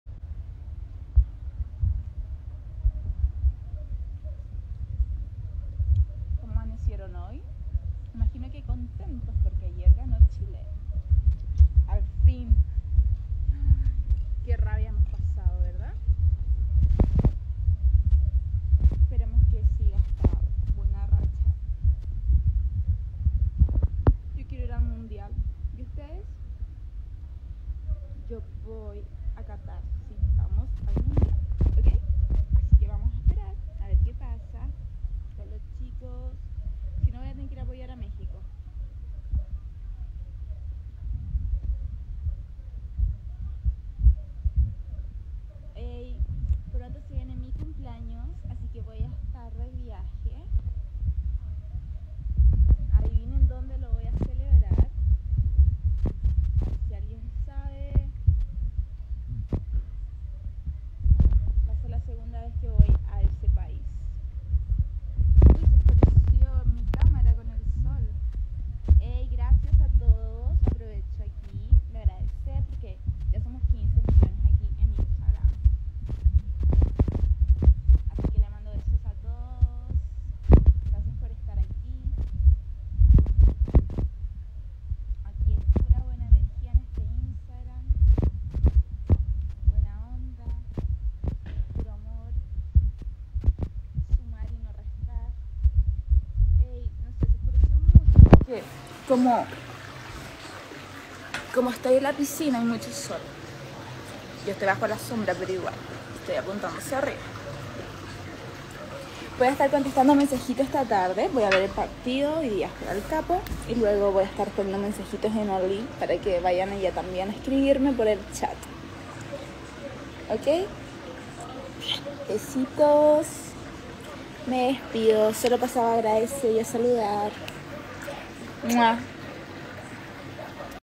Daniella Chavez - Instagram Live 11 De Octubre De 2021
Grabación integra de la transmisión que hizo desde su cuenta oficial de instagram la actriz y modelo chilena Daniella Chavez el pasado 11 de octubre de 2021.